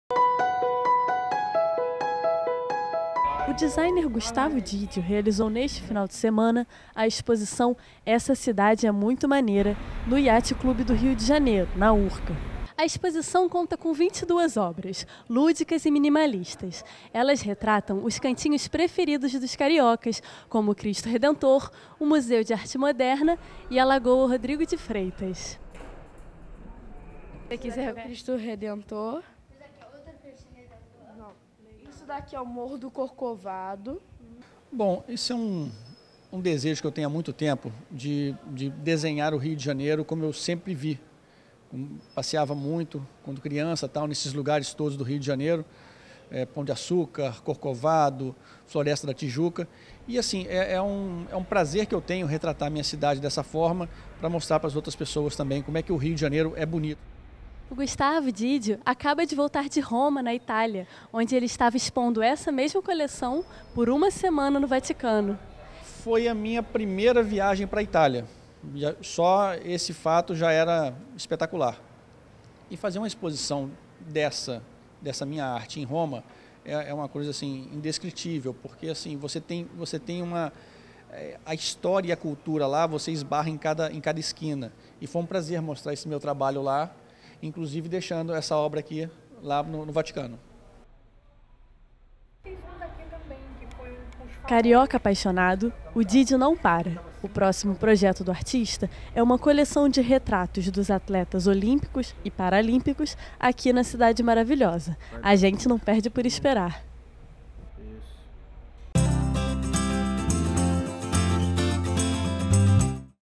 Exposição "Essa Cidade é Muito Maneira", de Gustavo Gigio
O designer carioca Gustavo Gigio, apresentou no último fim de semana, sua série "Essa Cidade é Muito Maneira", no Iate Clube do Rio de Janeiro, depois de uma rápida temporada em Roma.